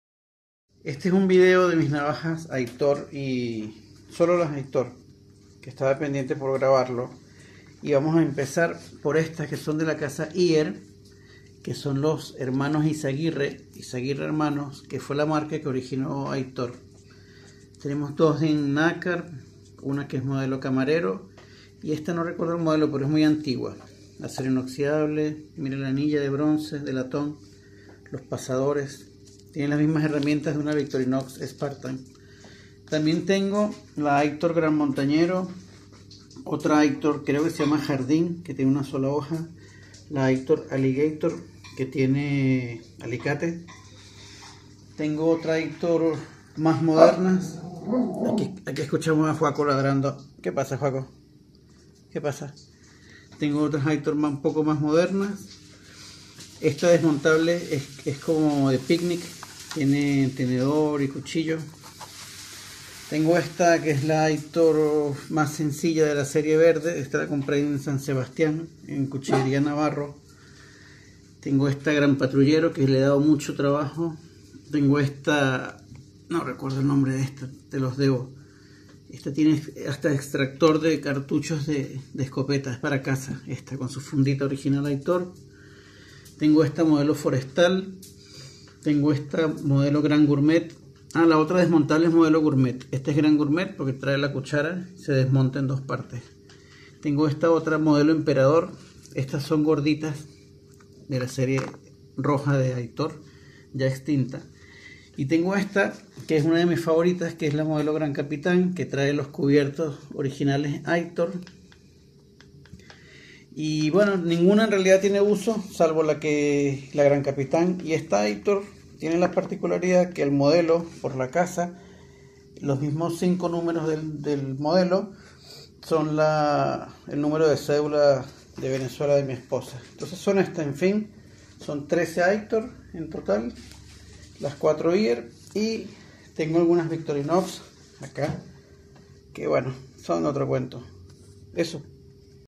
Colección navajas AITOR e IHER (Izaguirre Hermanos) Álvaro Sánchez (Chile)
Colección navajas AITOR e IHER (Izaguirre Hermanos)
REKADISTA MOTODIÓGENES RIDER TXATARRISTA